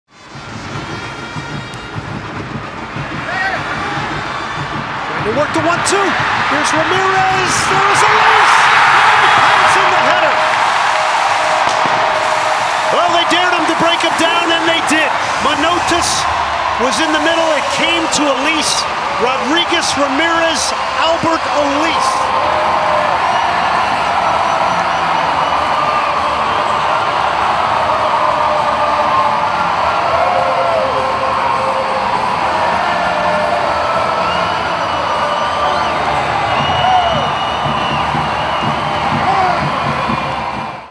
Alberth Elis heads it home | EVERY ANGLE
Assisted by a cross from Christian Ramirez, Alberth Elis scores his 8th goal of the season against Orlando City SC.